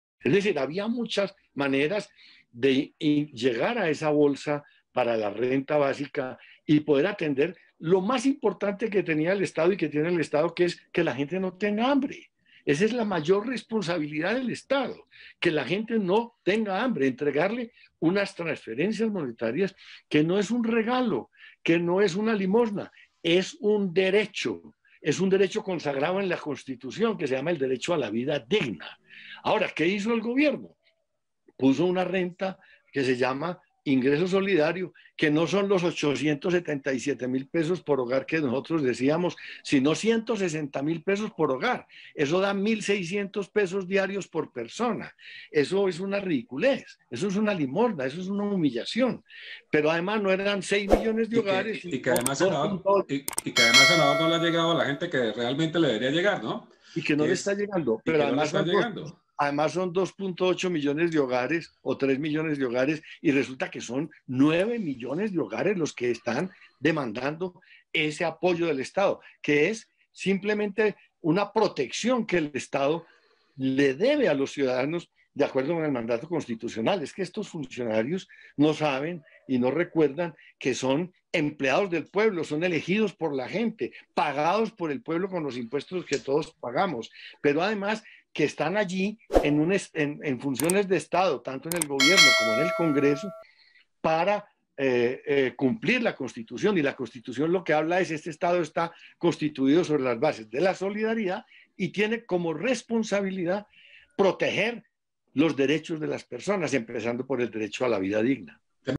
¿Qué paso con la renta básica? ¿Presidente Duque Responde?AbcBeneficios
►¿Problemas con el pago del incentivo? Dejame un comentario
Página de ingreso solidario
👉Página para consultar pago ed familias en acción.